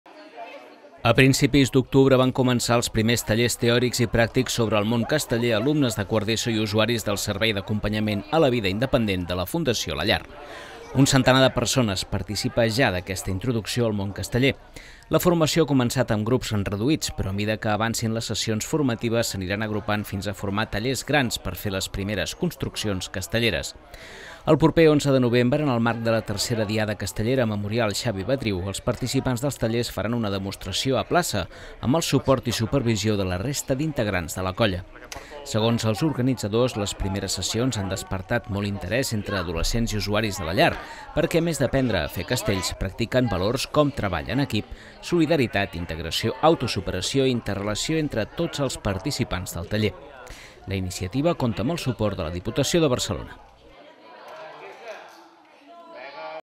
Els Castellers de Berga s’obren a tothom
Els Castellers de Berga van presentar divendres el projecte inclusiu ‘Castells amb tothom’. L’entitat col·labora amb l’Institut Guillem de Berguedà i la Fundació la Llar per acostar el món casteller i la cultura popular a adolescents i persones amb diversitat funcional a través de tallers i sessions formatives.